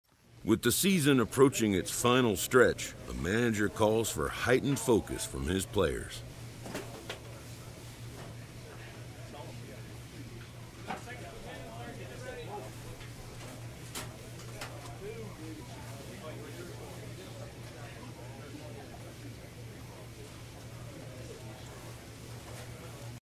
Postseason Push
The D'Backs gear up for the final stretch to make their push to the Postseason